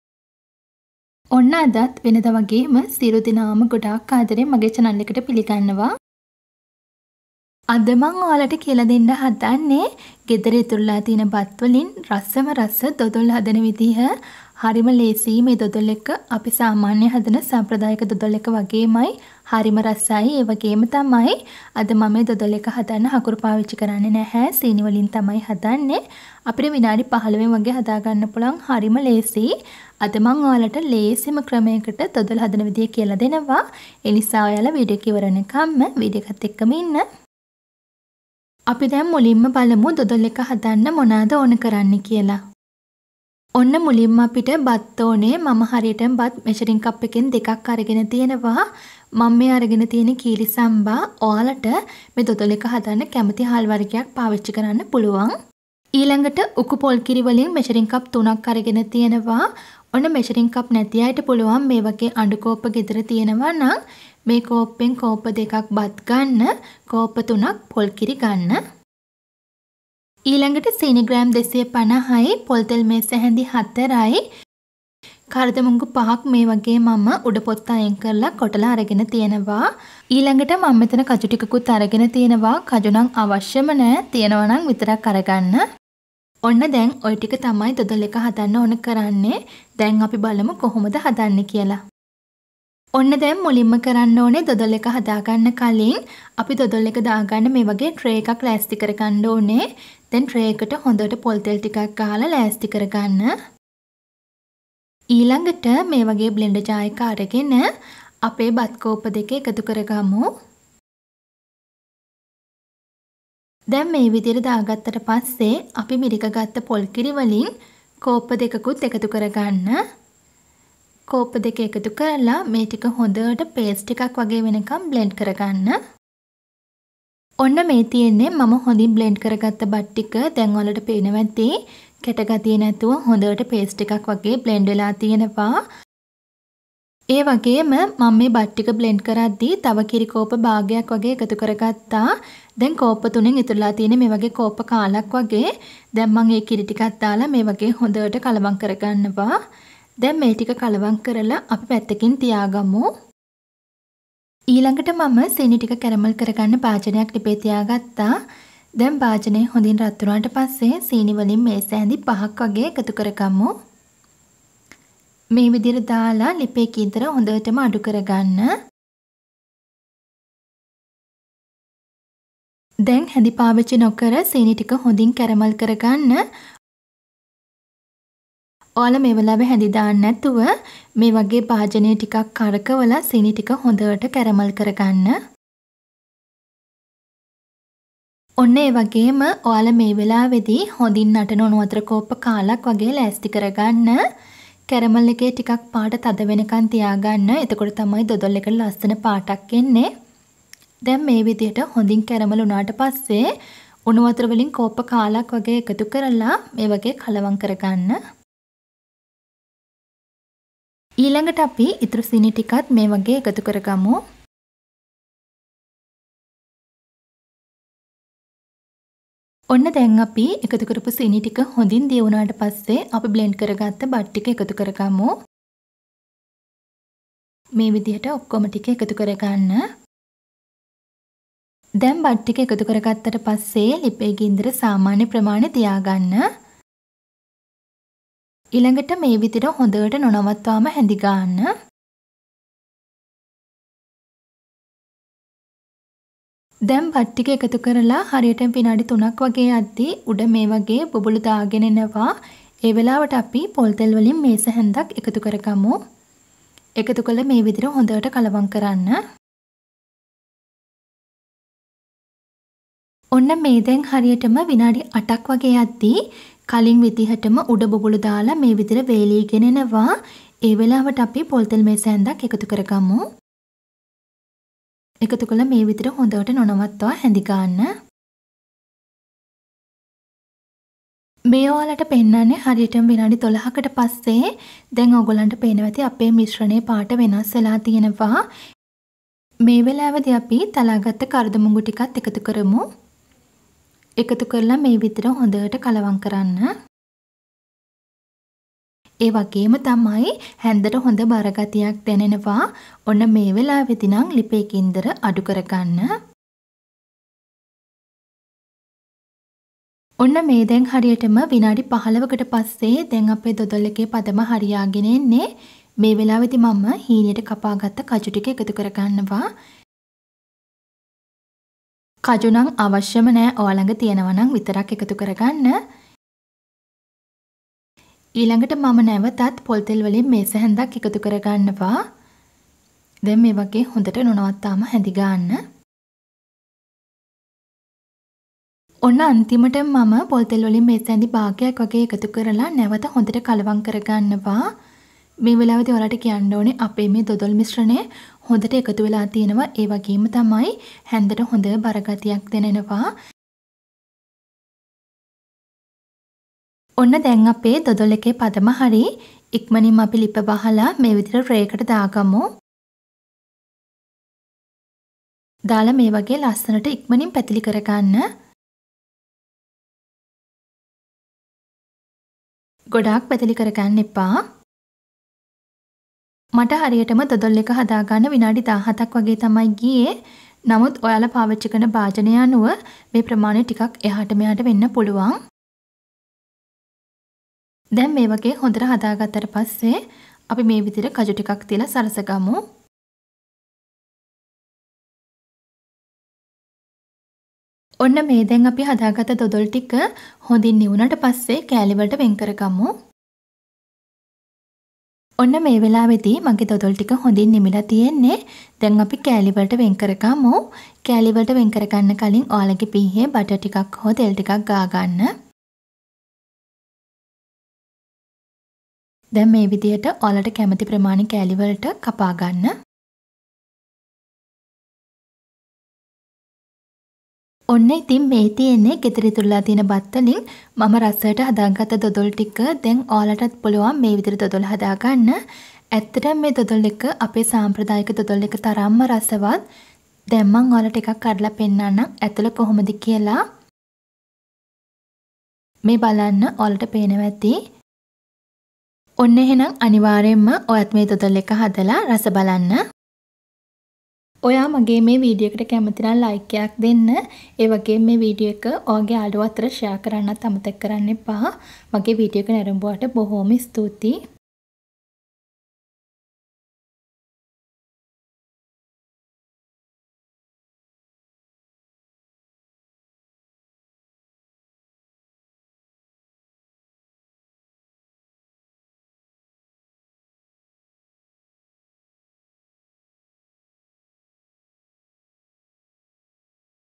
බත් වලින් විනාඩි 15න් කළු දොදොල් හදමු|Dodol recipe sinhala💓M.R KICHEN💓
අවශ්‍ය ද්‍රව්‍ය
බත් මෙෂරින් කප් දෙකක්
උකු පොල්කිරි මෙෂරින් කප් තුනක්
සීනි 250g
පොල් තෙල් මේස හැදි 4ක්
කරද මුංගු 5ක්